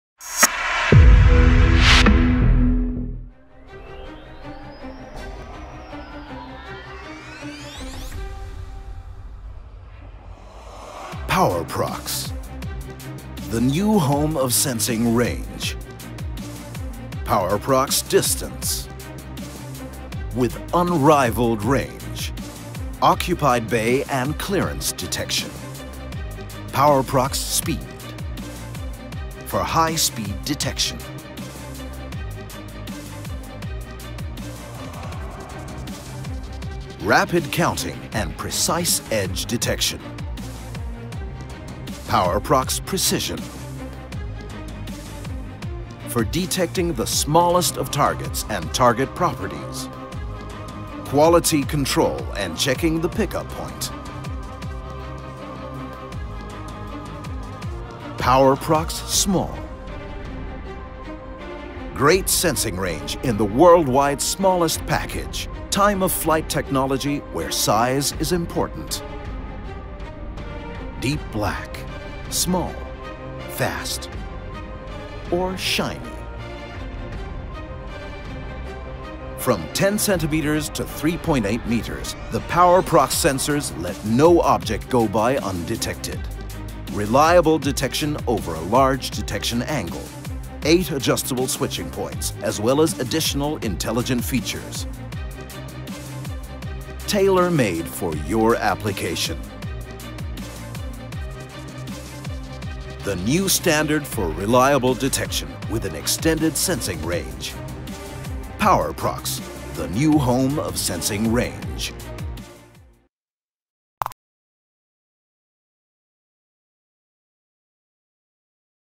SICK | PowerProx 光電感測器 | 不可思議的感測範圍
他是光電感測器界的強者，擁有無人能及的感測範圍。最遠可達3.8 公尺的檢測範圍。不論是深色、小型、高速移動甚至是高亮面的物體，皆無所遁形的完全感測，毫不錯失...更多產品資訊，請上網：